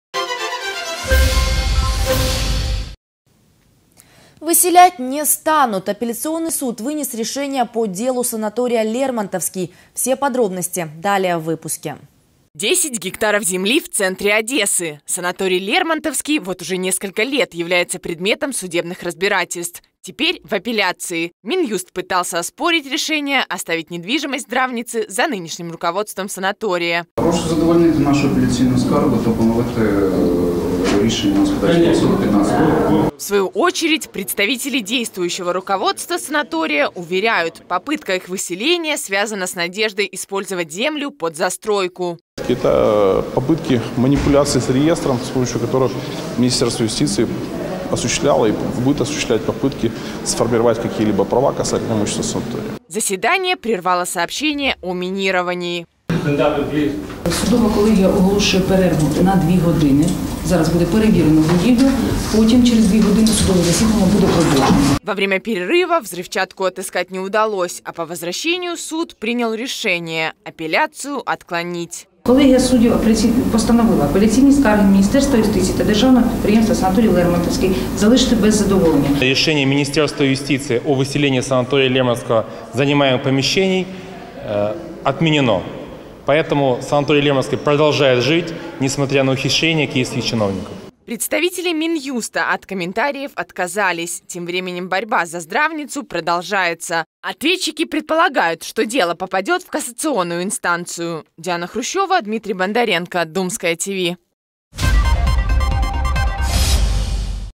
Апеляционный суд вынес решение по делу санатория «Лермонтовский»
10 гектаров земли в центре Одессы. Санаторий «Лермонтовский» вот уже несколько лет является предметом судебных разбирательств. Теперь в апелляции. Минюст пытался оспорить решение...